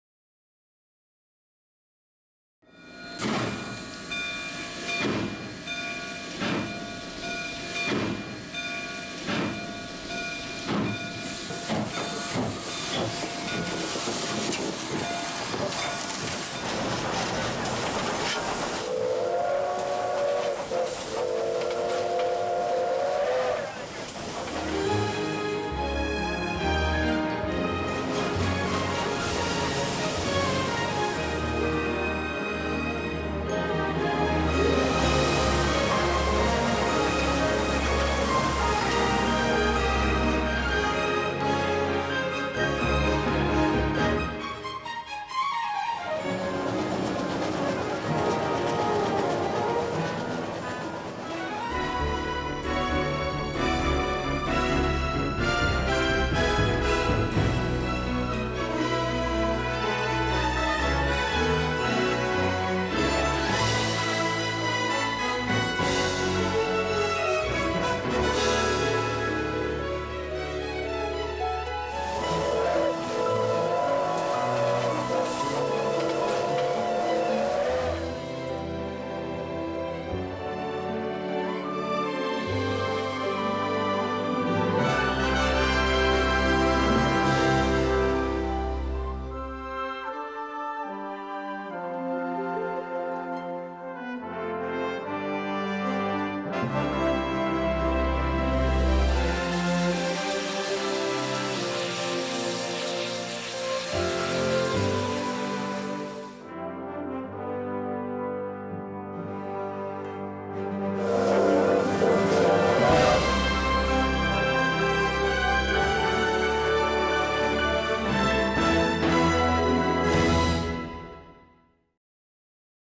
Steamtown train excursion to Tunkhannock Creek Viaduct (Nicholson Bridge) [2007]
Enjoy a ride from Steamtown to the Tunkhannock Creek Viaduct (Nicholson Bridge) in 2007. Edited by Larry Lavelle, former WNEP photographer/Home & Backyard producer.